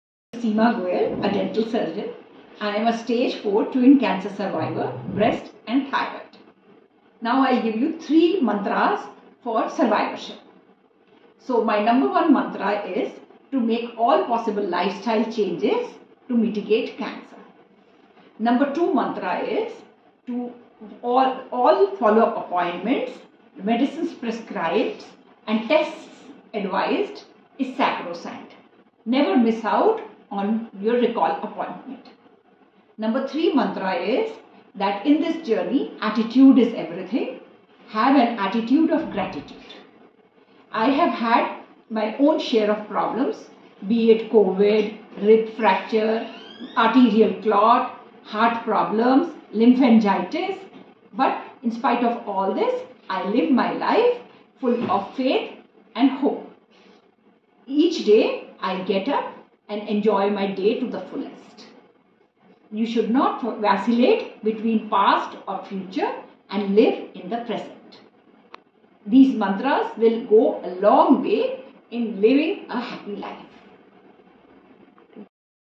You Beat Cancer by How you Live, Why you Live and the Manner in which you Live
This world cancer day, Dental surgeon, Dr. Seema Goyal shares her story and tells us her magical mantras of fighting and winning against this disease.

Our survivors are our knights in shining armours.